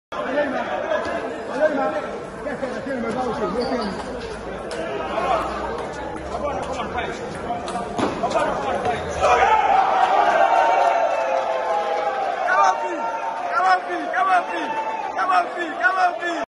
Ghana Table Tennis | Lord Kinful Vs Joshua Okine
The new form of celebration after a high lob